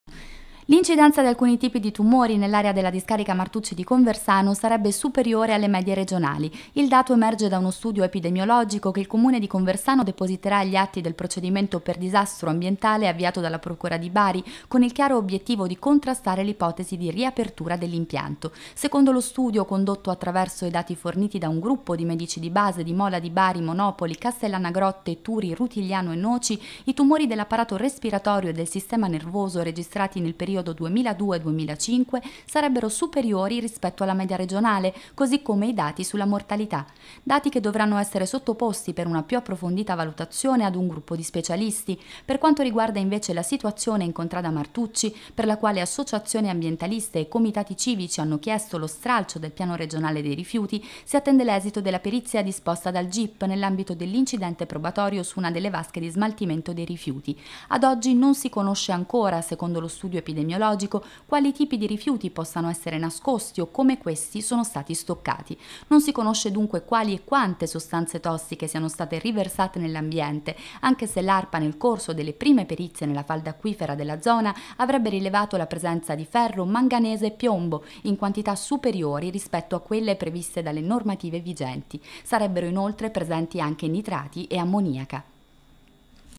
TG 23.10.13 Discarica Martucci, allarme tumori nei comuni del sud-est barese
Allarme tumori a Conversano e nei comuni limitrofi del sud-est barese. Secondo
i dati raccolti da un gruppo di medici di famiglia, nella zona della discarica
Martucci, i tumori dell'apparato respiratorio e del sistema nervoso sarebbero
aumentati.